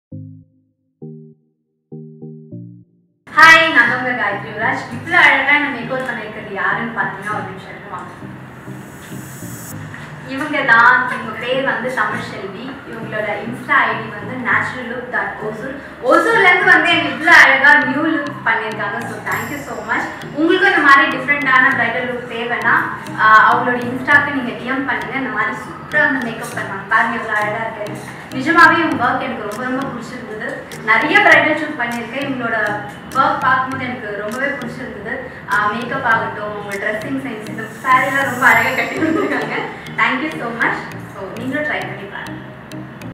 Gayathri Yuvraj Tamil serial actress bridal photoshoot by Naturallook Bridal Studio
South Indian Muhurtham Bridal Makeup Tutorial | Hair and Makeup | Naturallook Bridal Studio | Hosur.